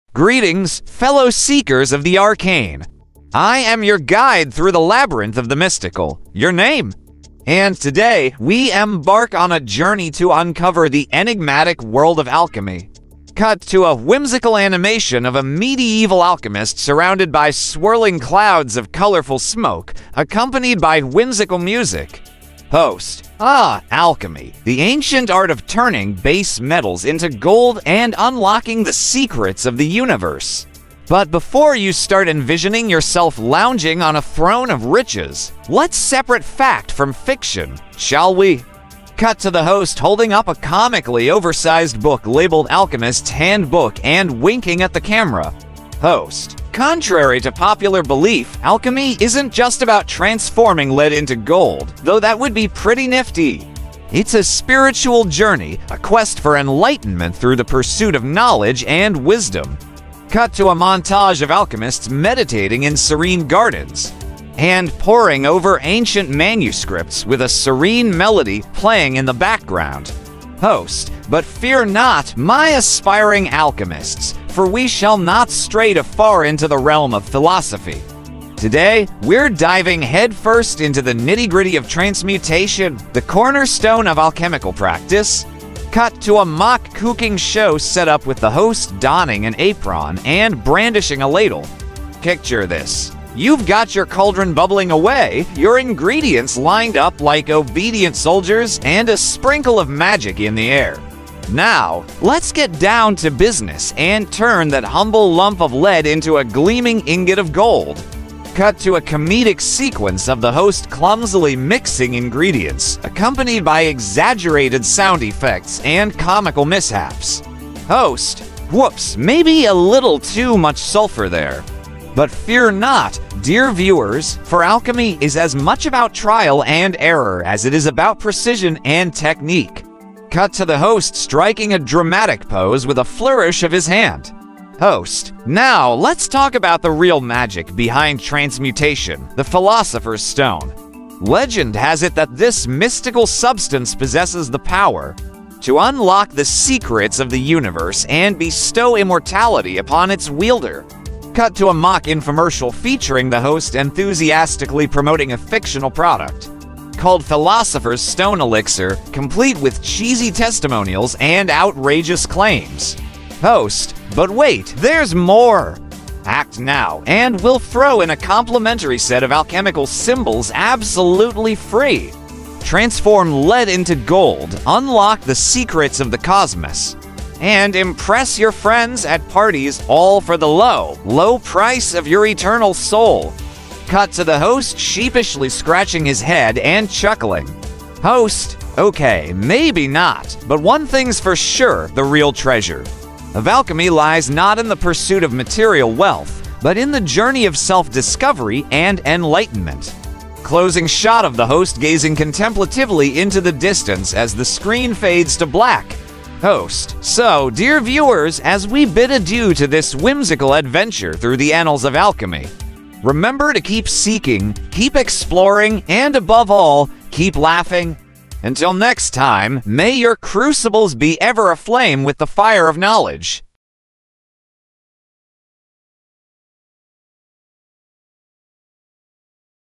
The Alchemist's Guide to Precious Metals: Unveiling the Secrets of Transmutation
(BONUS VIDEO) Element Uncovered explores the world of alchemy, debunking myths while celebrating its mystical allure. Hosted by [Your Name], it emphasizes alchemy's spiritual journey and the pursuit of knowledge through comical demonstrations and exaggerated claims about the Philosopher's Stone.